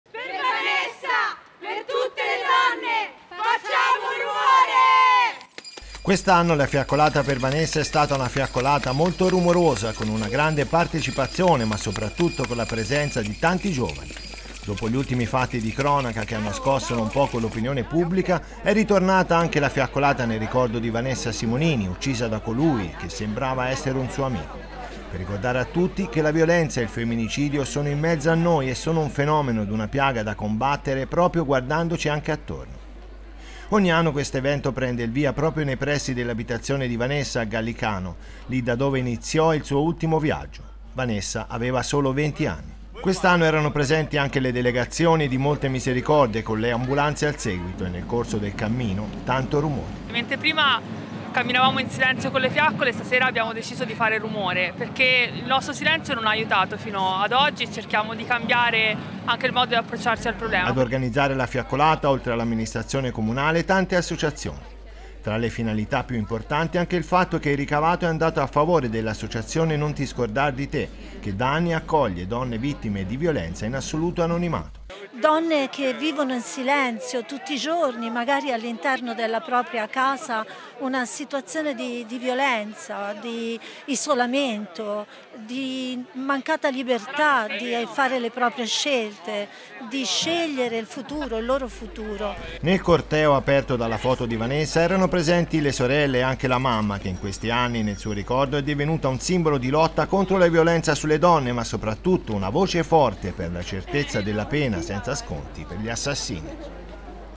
In centinaia alla marcia per ricordare Vanessa Simonini a Gallicano
Quest’anno la fiaccolata per Vanessa Simonini è stata molto rumorosa con una grande partecipazione, soprattutto di giovani.  Dopo gli ultimi fatti di cronaca che hanno scosso l’opinione pubblica, è tornato anche il tradizionale appuntamento nel ricordo della giovane uccisa da colui che sembrava essere un suo amico